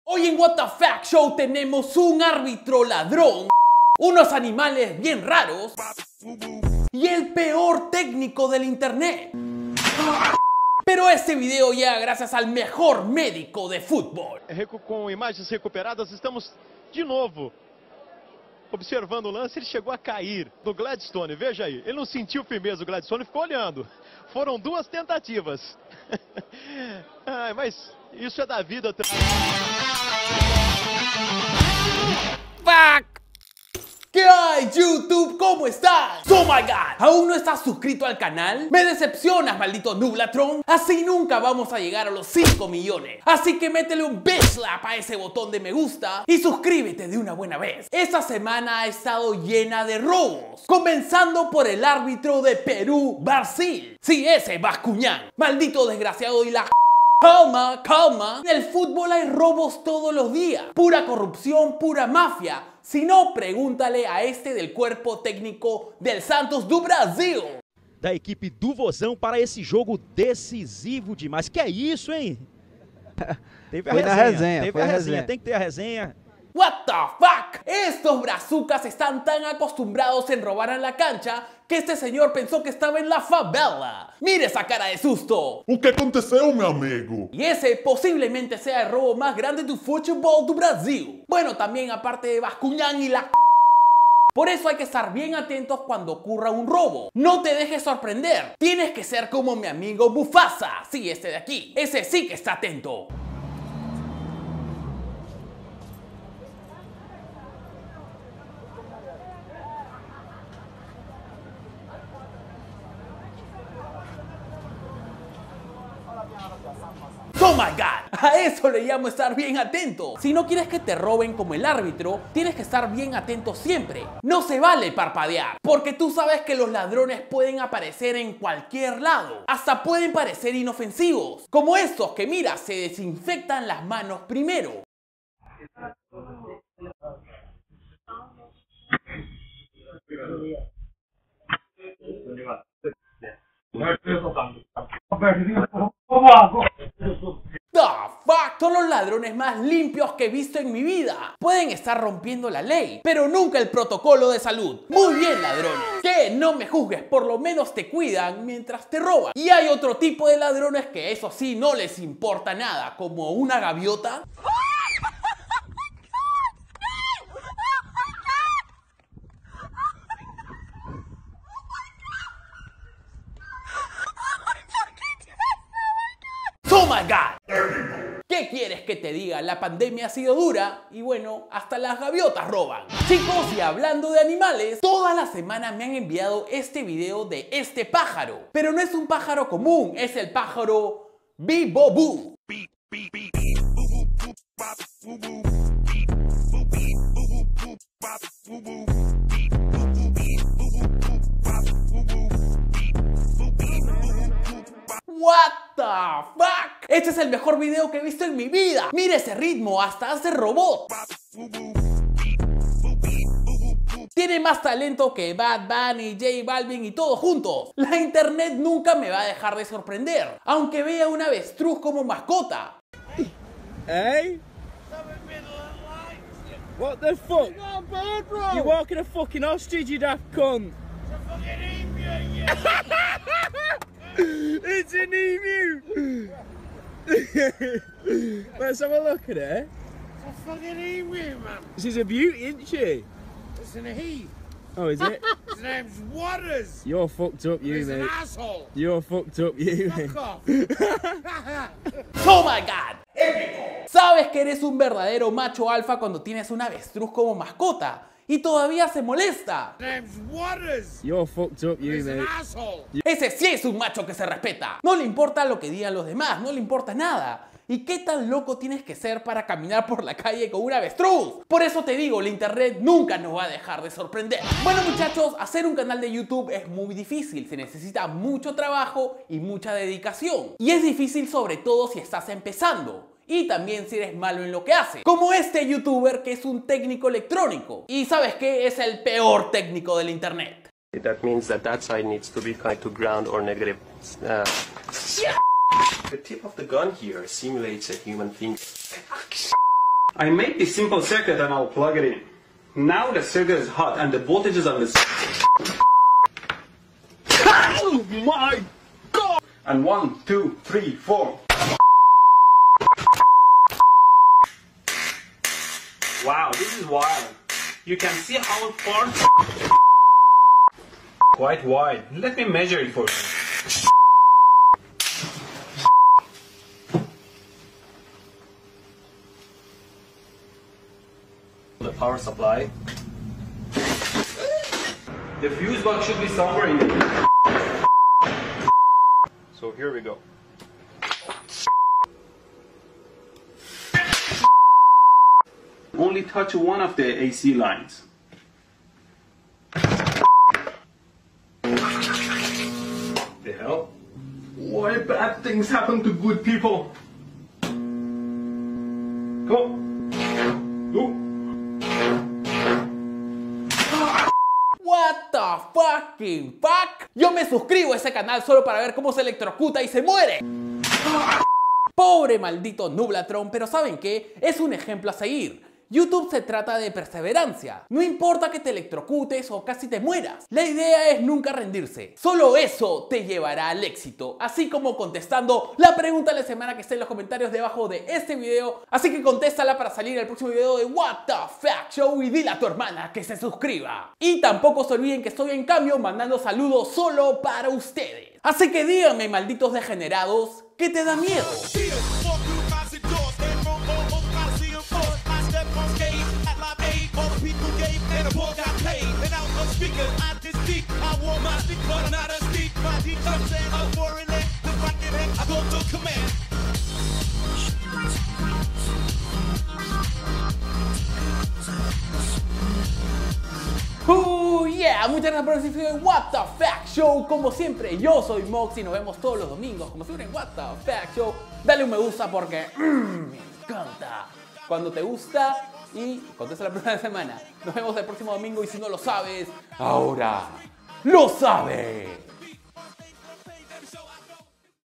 El Robo Mas Grande del Futbol!
Si crees que bascuñan es el mas ladron... Tienes que ver este video! El Robo Mas Grande del Futbol!

Fuentes de Video:

Jukin Media
Elrellano.com
Break
Viraltrend
LiveLeak